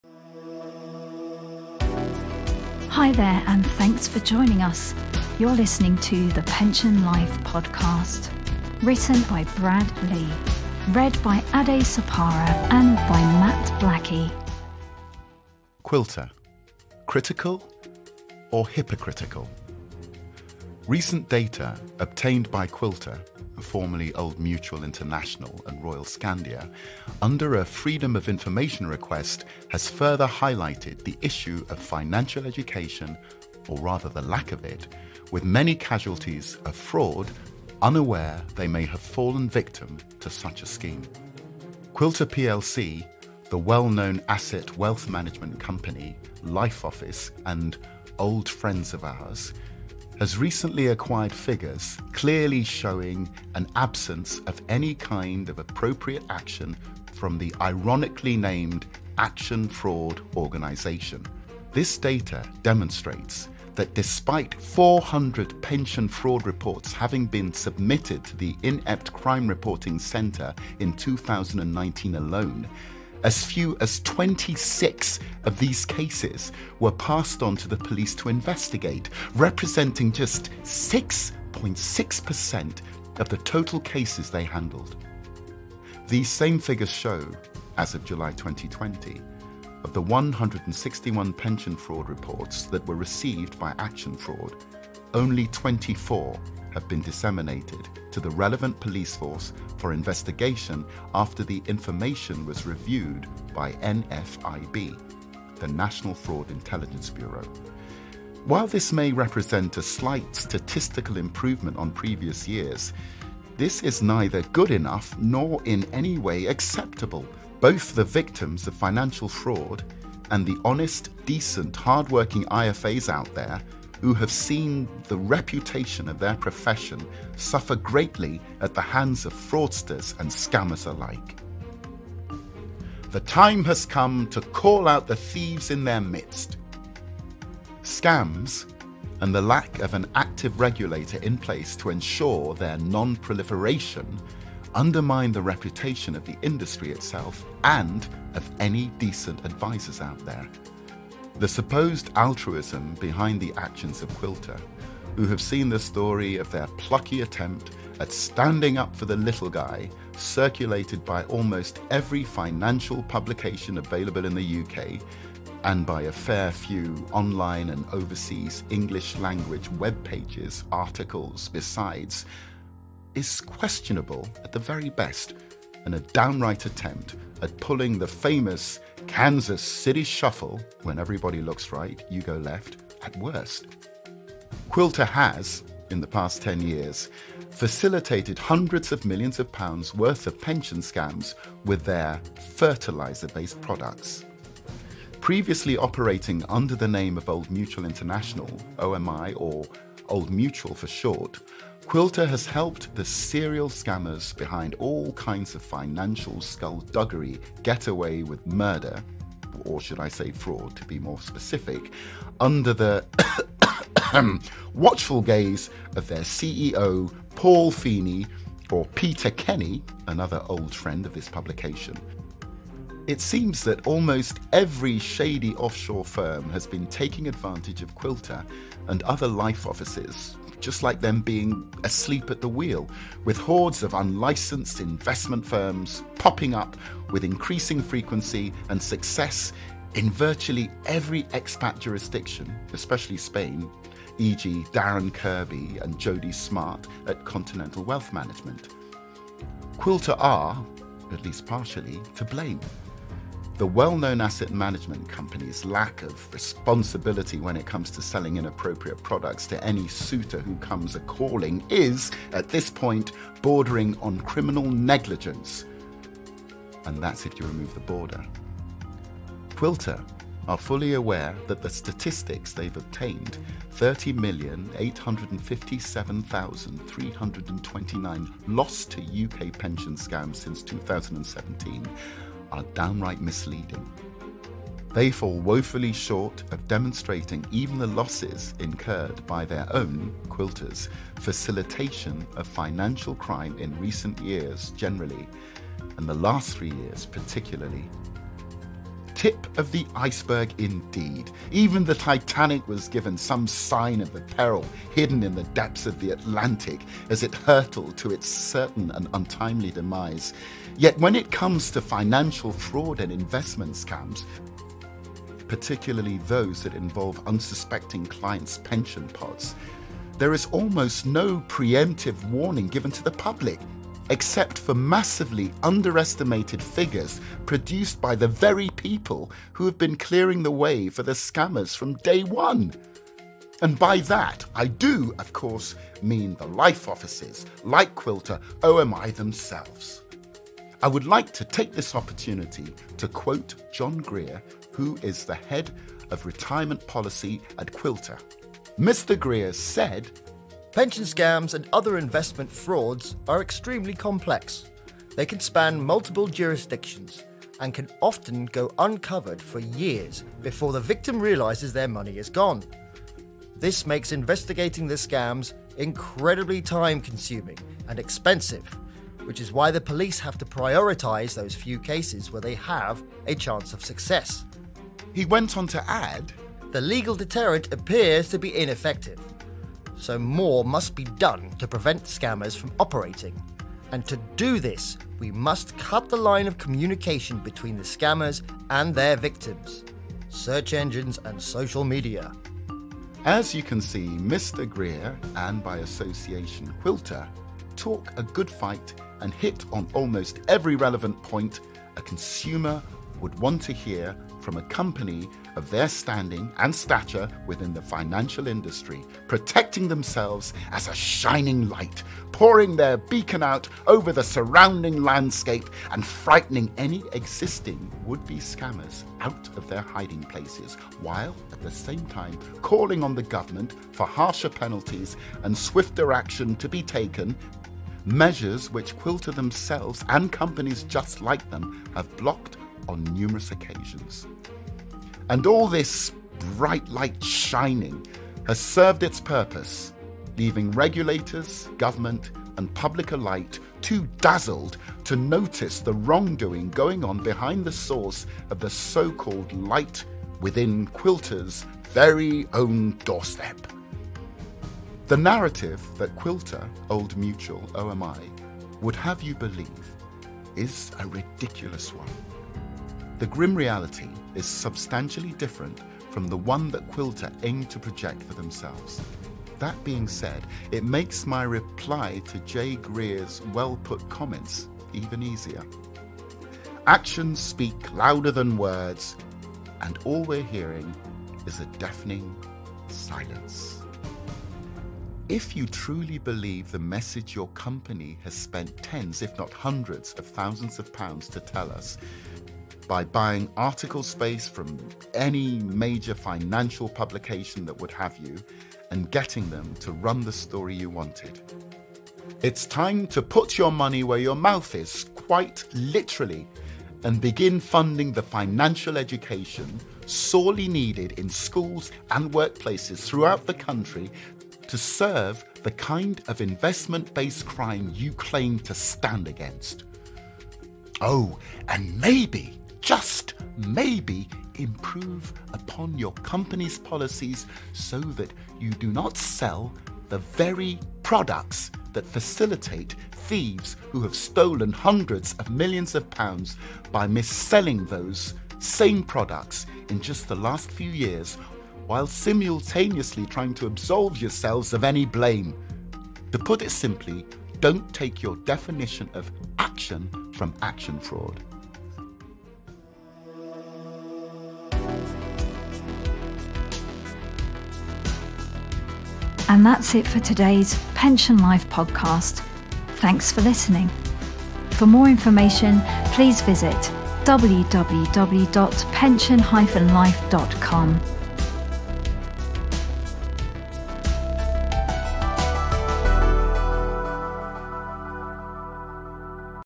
Quilter: Critical or Hypocritical?
Quilter International - formerly Old Mutual International - has talked about victims of fraud, but without mentioning that Quilter itself is a major cause and facilitator of investment and pension fraud.  Giving terms of business to unlicensed scammers (for the past decade), Quilter has stood by while the scammers sold insurance bonds illegally and then invested the victims' life savings and pensions in the worthless, toxic rubbish unregulated funds and structured notes they offer on their platforms for the scammers to use.  Quilter has drawn attention to the fact that the hopelessly inept Action Fraud organisation in the UK only referred 6% of all fraud reports to the Police in 2019.  This, of course, means that the perpetrators and facilitators repeatedly get away with fraud and the Police do nothing about it.  So, of course, Quilter - and all the other death offices who peddle pointless and expensive insurance bonds that nobody needs or can afford - continue to get away with ruining thousands of innocent victims.  Of course, the ethical sector of financial services - such as the majority of UK-based financial advisers who are licensed and qualified - pay the price.  Their reputation and consumer confidence in the industry and the profession suffer terribly as a result.  Until death offices such as Quilter are shut down, this fraud will simply continued unabated and unlimited of further victims will lose their retirement savings unnecessarily.